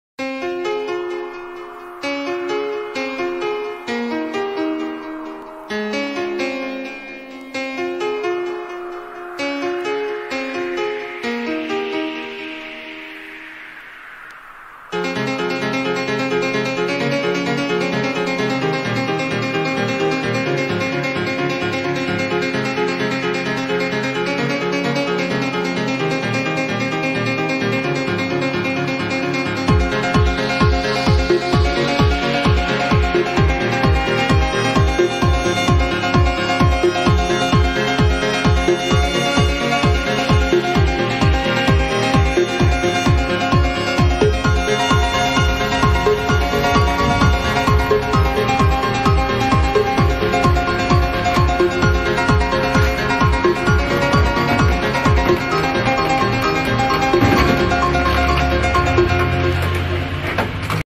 ACCIDENT; KIA Wagon Smashes Ford E350 Hits Tree
RESPONSE Towing on scene. (Towing & Recovery) for the footage.